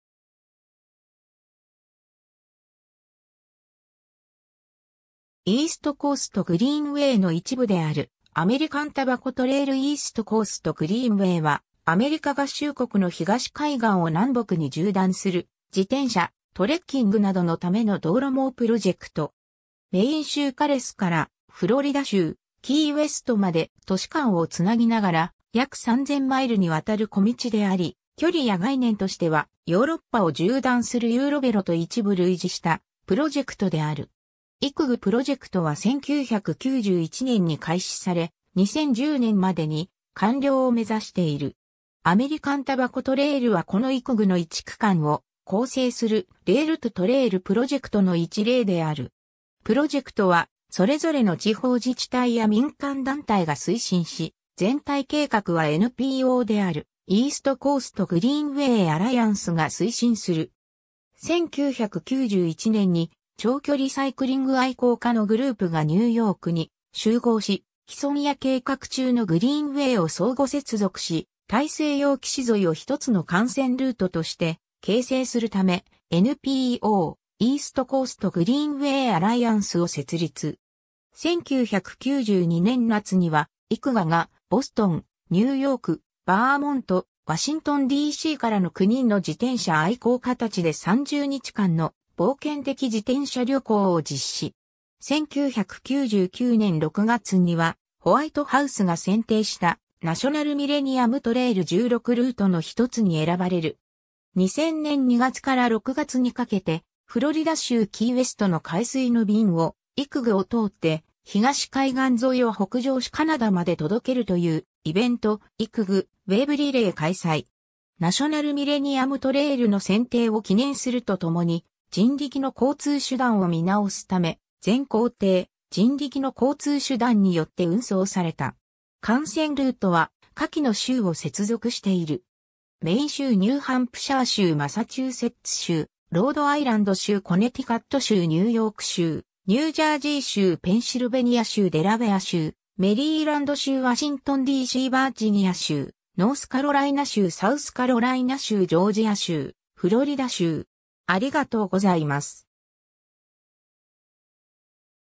イーストコースト・グリーンウェイ
イーストコースト・グリーンウェイの一部であるアメリカン・タバコ・トレイル イーストコースト・グリーンウェイ（East Coast Greenway、略称ECG）はアメリカ合衆国の東海岸を南北に縦断する、自転車、トレッキングなどのための道路網プロジェクト。
メイン州カレスからフロリダ州キーウェストまで、都市間を繋ぎながら、約3,000マイル（4,828km）に亘る小道であり、距離や概念としては、ヨーロッパを縦断するユーロヴェロと一部類似したプロジェクトである。
 ECGプロジェクトは1991年に開始され、2010年までに完了を目指している。
アメリカン・タバコ・トレイルはこのECGの一区間を構成する「レイル・トゥ・トレイル（鉄道の廃線区間をコースにしたトレイル）」プロジェクトの一例である。
プロジェクトはそれぞれの地方自治体や民間団体が推進し、全体計画はNPOである「イーストコースト・グリーンウェイ・アライアンス（ECGA）」が推進する。
 1991年に長距離サイクリング愛好家のグループがニューヨークに集合し、既存や計画中のグリーンウェイを相互接続し、大西洋岸沿いを一つの幹線ルートとして形成するため、NPO「イーストコースト・グリーンウェイ・アライアンス（ECGA）」を設立。
 1992年夏には、ECGAがボストン、ニューヨーク、バーモント、ワシントンD.C.からの9人の自転車愛好家たちで30日間の「冒険的」自転車旅行を実施。
 1999年6月には、ホワイトハウスが選定した「ナショナル・ミレニアム・トレイル」16ルートの一つに選ばれる。
 2000年2月から6月にかけて、フロリダ州キーウェストの海水の瓶をECGを通って東海岸沿いを北上しカナダまで届けるというイベント「ECGウェーブ・リレー」開催。
「ナショナル・ミレニアム・トレイル」の選定を記念するとともに、人力の交通手段を見直すため、全行程、人力の交通手段によって運送された。
 幹線ルートは下記の州を接続している。
 メイン州 ニューハンプシャー州 マサチューセッツ州 ロードアイランド州 コネティカット州 ニューヨーク州 ニュージャージー州 ペンシルベニア州 デラウェア州 メリーランド州 ワシントンD.C. バージニア州 ノースカロライナ州 サウスカロライナ州 ジョージア州 フロリダ州